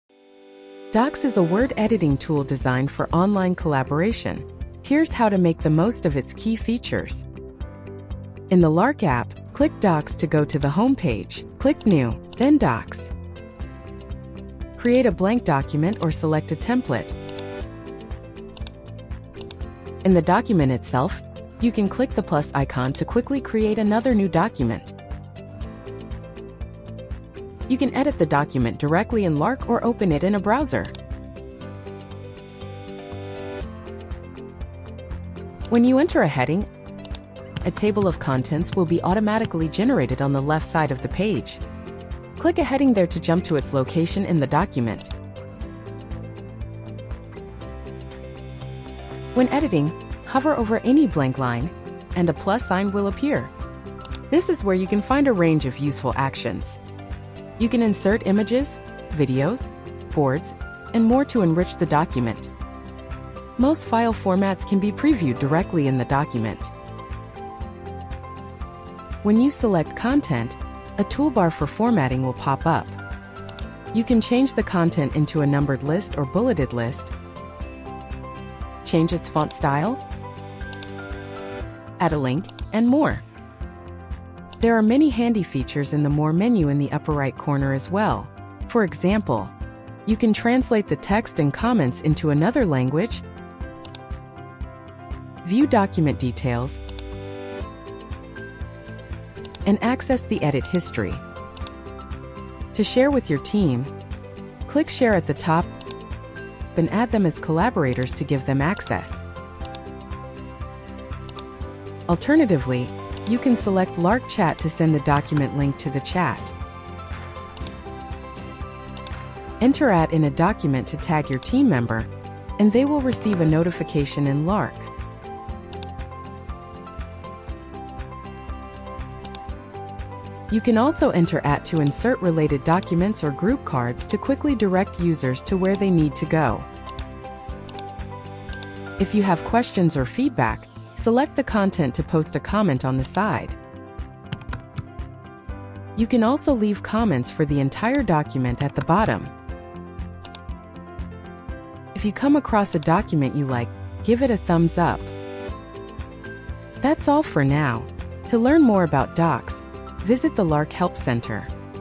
Get started with Lark Docs | Best Real-Time Document Collaboration Tools
Discover more about our features and offerings on the official Lark

Lark is an all-in-one productivity superapp trusted by teams of all sizes. In this video, we will guide you through Lark Docs, a powerful real-time document collaboration tool. Learn how to create, edit, and collaborate on documents with your team—whether you’re managing creative projects, agile workflows, or daily tasks.

Try Lark for free and find the perfect plan for you

00:00 Intro to Lark Docs
00:07 Create a document
00:27 Edit and format text
01:43 Collaborate in real-time